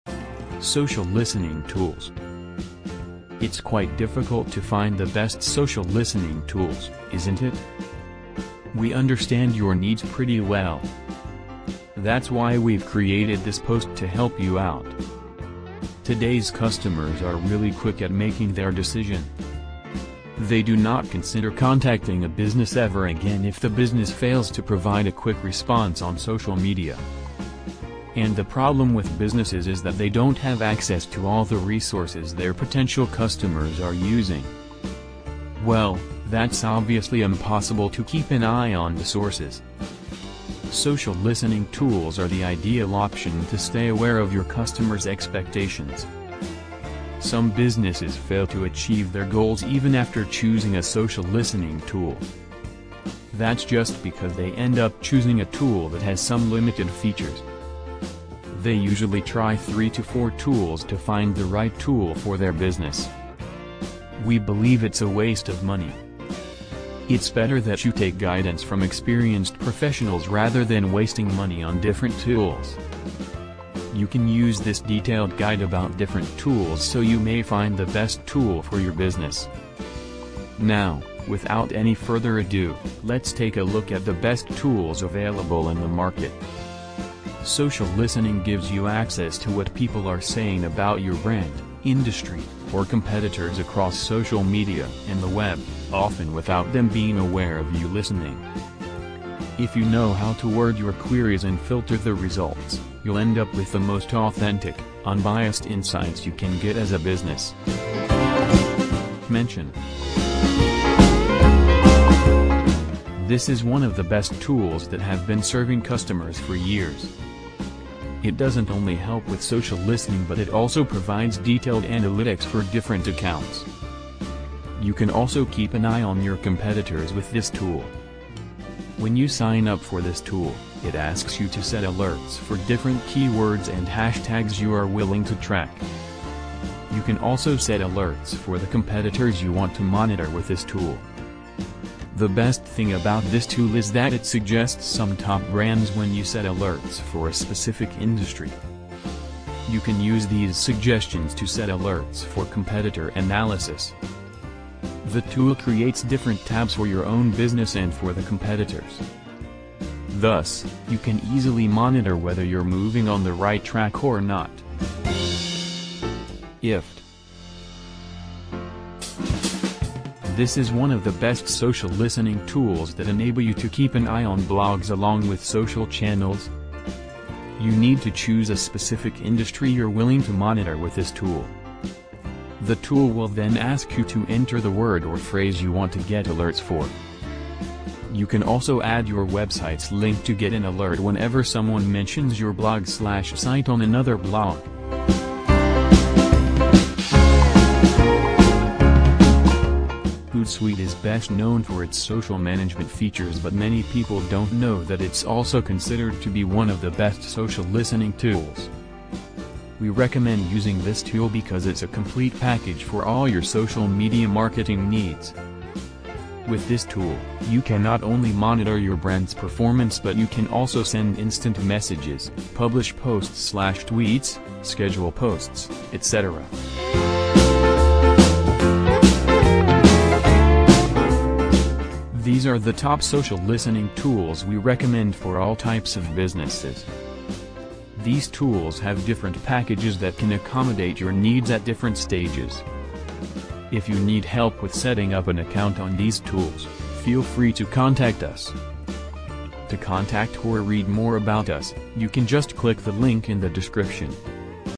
Social Listening Tools
It’s quite difficult to find the best social listening tools. Isn’t it?

We understand your needs pretty well. That’s why we’ve created this post to help you out. Today’s customers are really quick at making their decision. They do not consider contacting a business ever again if the business fails to provide a quick response on social media.

And the problem with businesses is that they don’t have access to all the resources their potential customers are using. Well, that’s obviously impossible to keep an eye on the sources. Social listening tools are the ideal option to stay aware of your customers’ expectations.